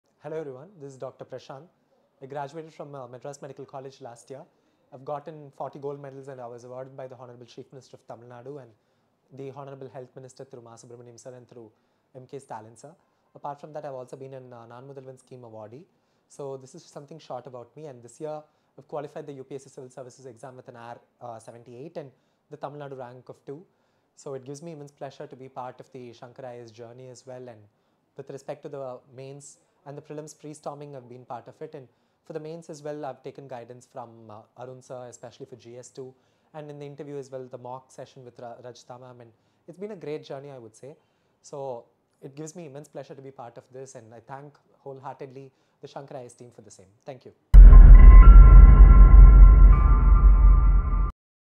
UPSC Topper Talk: Dr. S. Prashanth, AIR - 78 | UPSC CSE 2023
In this video of UPSC Topper Talk series, Dr. S. Prashanth who had secured All India Rank - 78 shared about his preparation journey.

We are proud to say that Dr. S. Prashanth, is a classroom program student of Shankar IAS Academy.

We congratulate him on behalf of Shankar IAS Academy family and we wish all the best for his future endeavours as a bureaucrat.